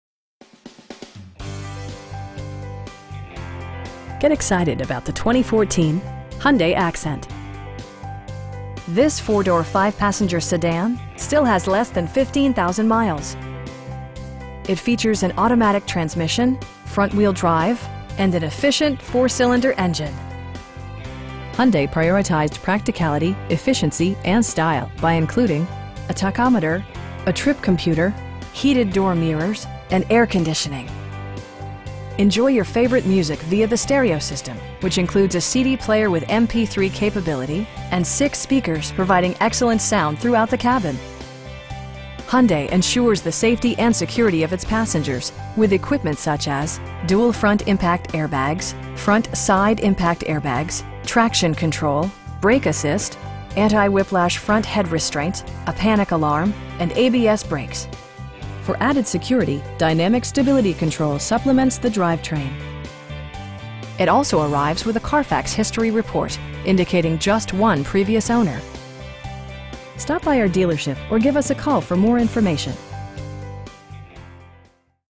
2014 Hyundai Accent GLS in Midland, TX 79703
All American Chrysler Jeep Dodge of Midland

Get excited about the 2014 Hyundai Accent. This 4 door, 5 passenger sedan still has less than 15,000 miles! It features an automatic transmission, front-wheel drive, and an efficient 4 cylinder engine. Hyundai prioritized practicality, efficiency, and style by including: a tachometer, a trip computer, tilt steering wheel, heated door mirrors, power windows, an overhead console, and air conditioning. Hyundai ensures the safety and security of its passengers with equipment such as: dual front impact airbags, front SIDE impact airbags, traction control, brake assist, anti-whiplash front head restraints, a panic alarm, and ABS brakes. For added security, dynamic Stability Control supplements the drivetrain. It also arrives with a Carfax history report, indicating just one previous owner. We pride ourselves in consistently exceeding our customer's expectations. Stop by our dealership or give us a call for more information.